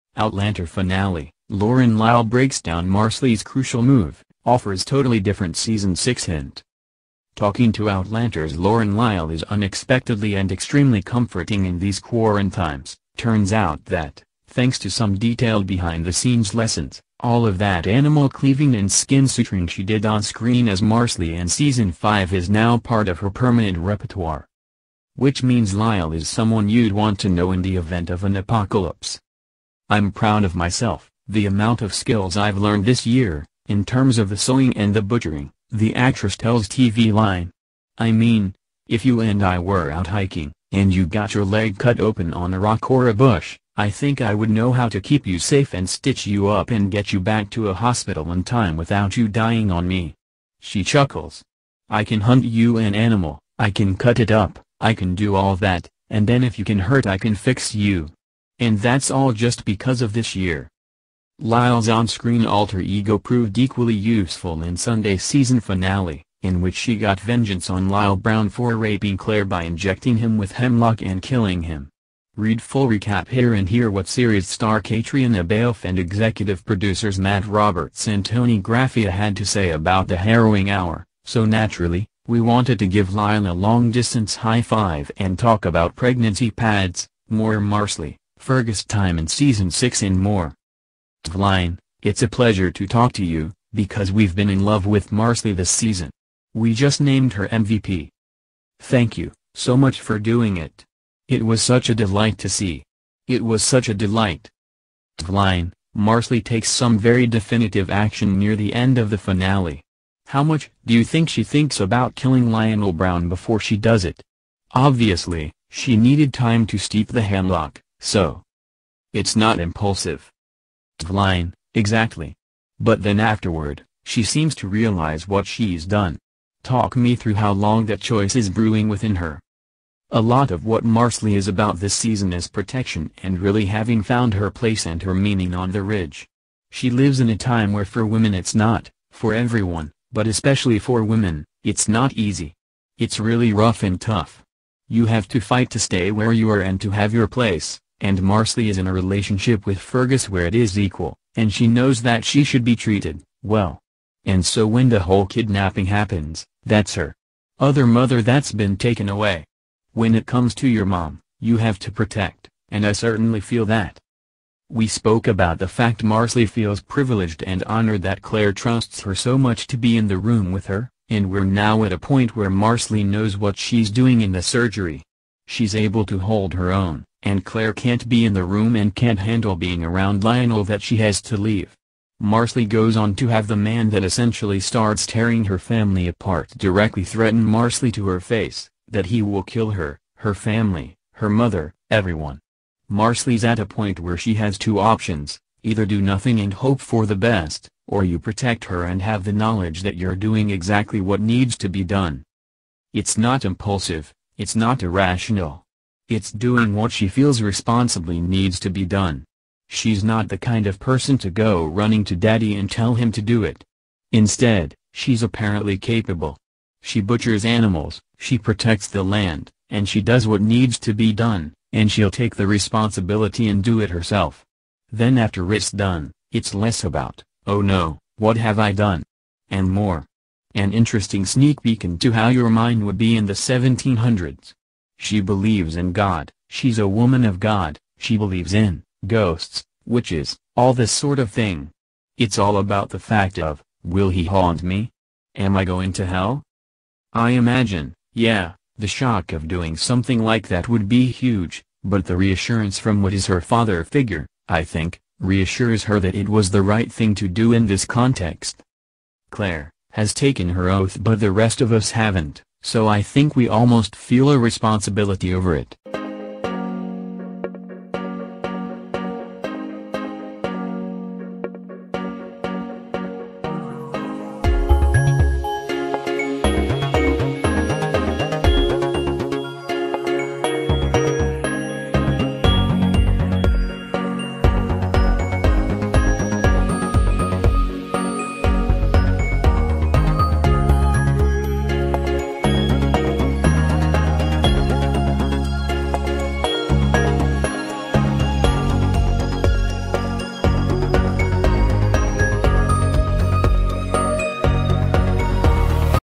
Outlander Finale: Lauren Lyle Breaks Down Marsali's Crucial Move, Offers 'Totally Different' S6 Hint
Outlander Finale: Lauren Lyle Breaks Down Marsali's Crucial Move, Offers 'Totally Different' Season 6 Hint